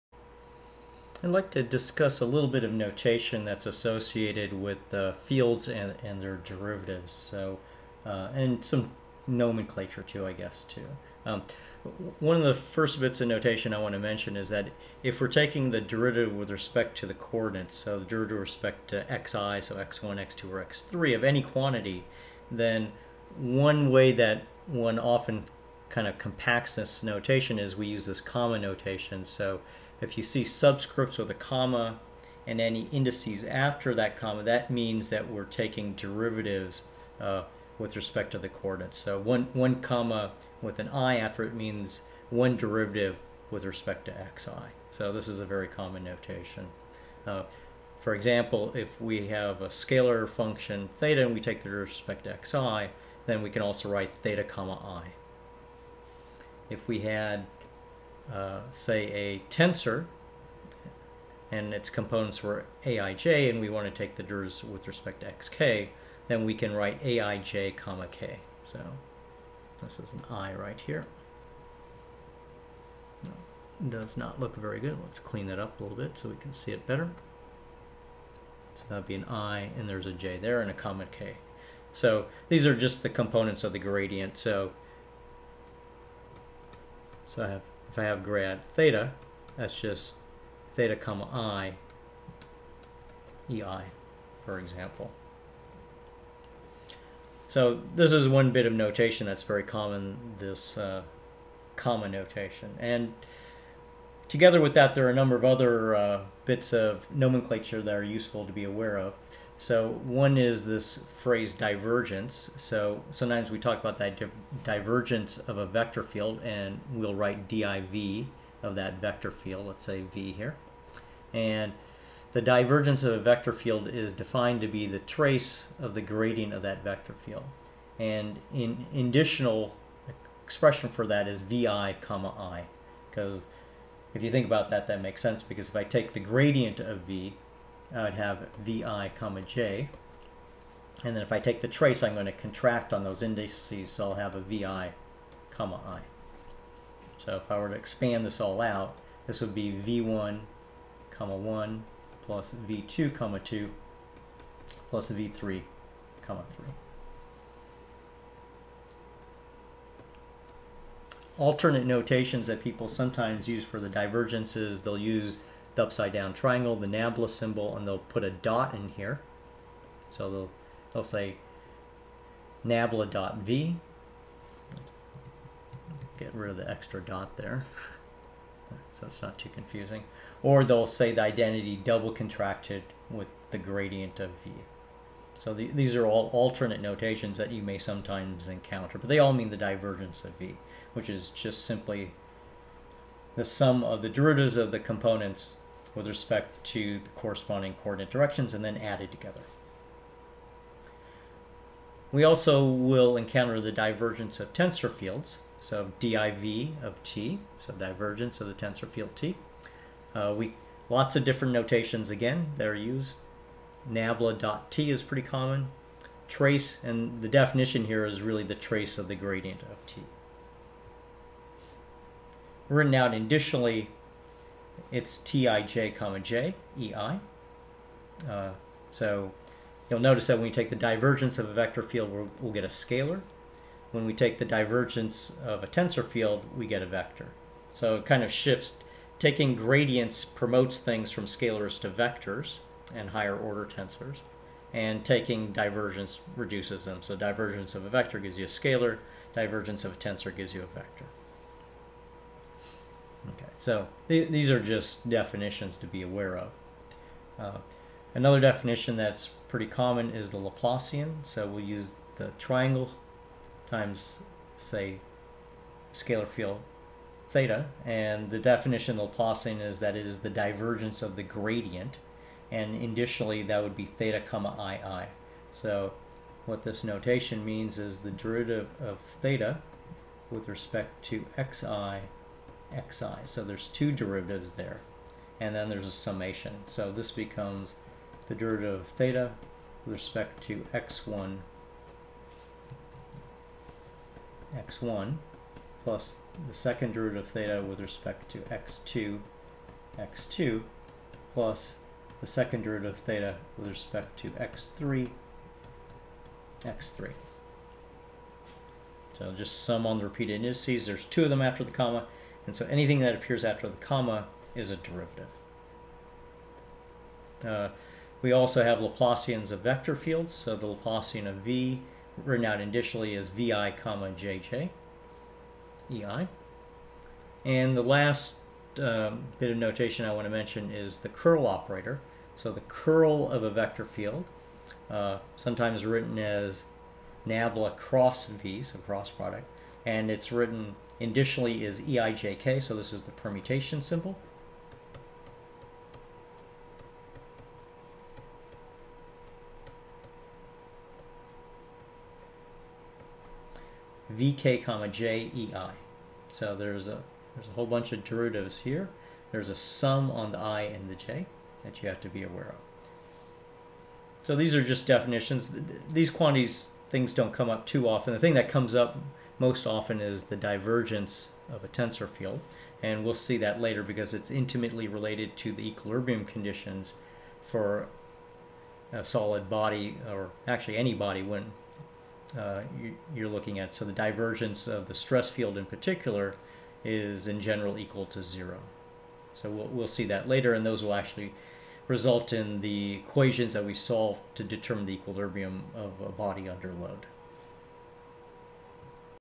5. Derivative notation and nomenclature
Discussion of derivative comma notation and nomenclature: divergence, curl, Laplacian of scalar, vector, and tensor fields